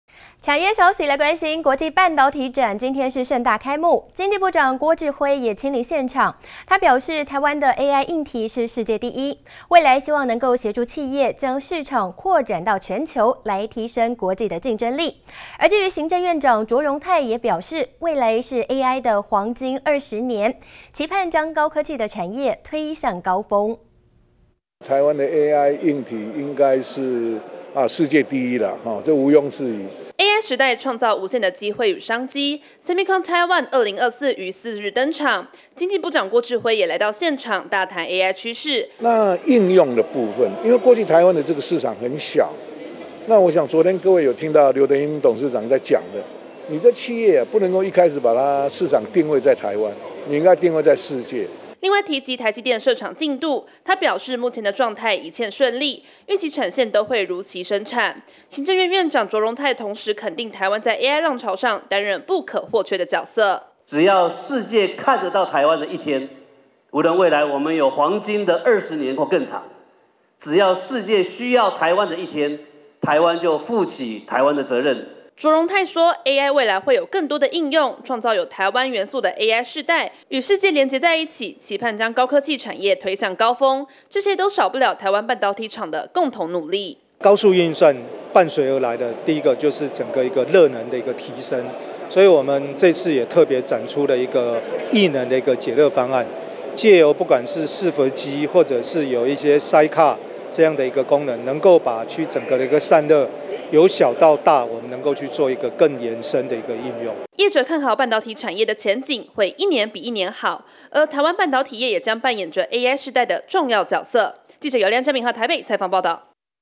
國際半導體展盛大登場 卓榮泰誓打造台灣元素AI時代! 經長承諾助小企業定位全球 談台積電設廠"進度順利"｜非凡財經新聞｜20240904
國際半導體展今(4)日盛大開幕，經濟部長郭智輝也親臨現場，表示台灣的AI硬體是世界第一，未來希望能協助企業，將市場擴展到全球，提升國際競爭力。行政院長卓榮泰也表示，未來是AI的黃金二十年，期盼將高科技產業推向高峰。